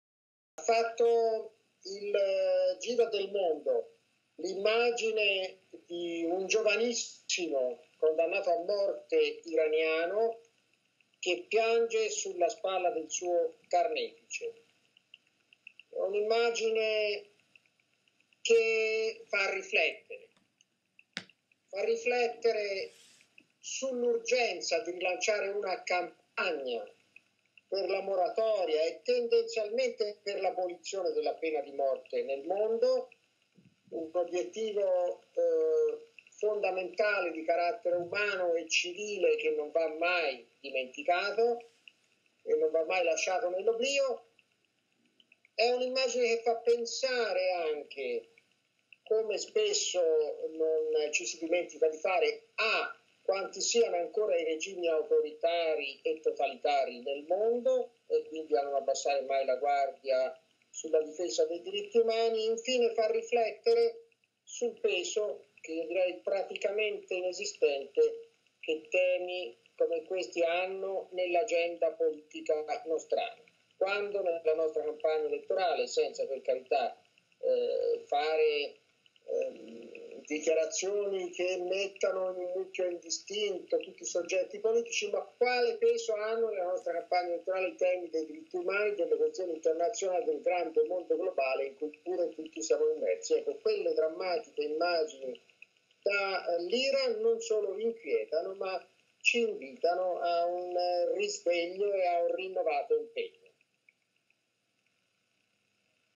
Contro la pena di morte, un impegno mancato
Severino Saccardi, direttore della rivista Testimonianze, commenta la drammatica immagine dei giovani condannati a morte in Iran per ribadire l'urgenza di una moratoria della pena di morte nel mondo e la colpevole assenza, nella politica italiana, di questo tema.